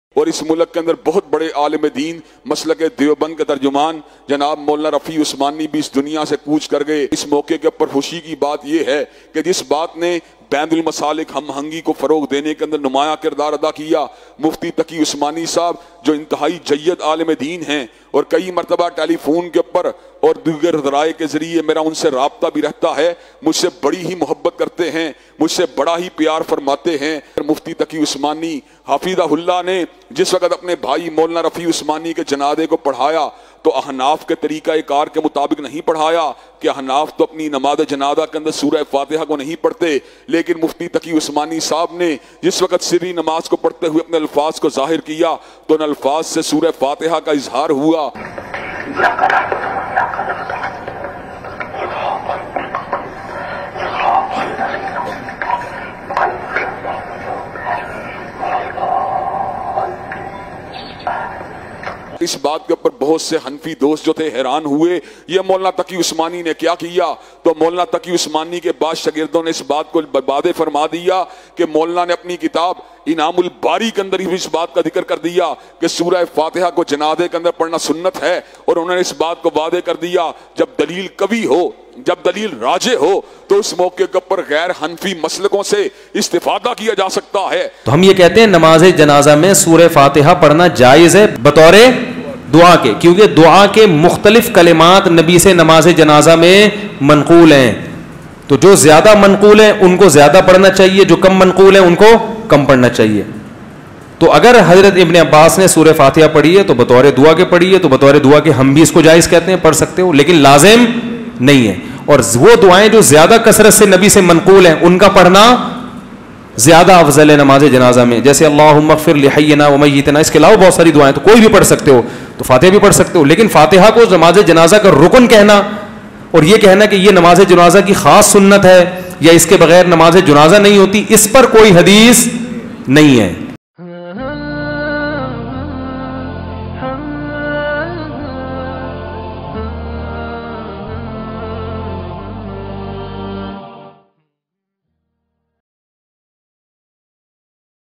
Mufti Taqi Usmani Sahab ny Namaz e Janaza main Surah Fatiha Kyu Parhi ? Allama Ibtisam Elahi Zaheer
Mufti Taqi Usmani ny Namaz e Janaza main Surah Fatiha Kyu Parhi ? Allama Ibtisam Elahi Zaheer

شیخ الاسلام مفتی تقی عثمانی نے مفتی رفیع عثمانیؒ کی نماز جنازہ میں فاتحہ کیوں پڑھی ؟
ایک اہل حدیث عالم علامہ ابتسام الہیٰ ظہیر کا مفتی تقی عثمانی کوخراج عقیدت
مفتی طارق مسعود صاحب نے نماز جنازہ میں سورۃ فاتحہ پڑھنے اور نہ پڑھنے کی وجہ بتائی۔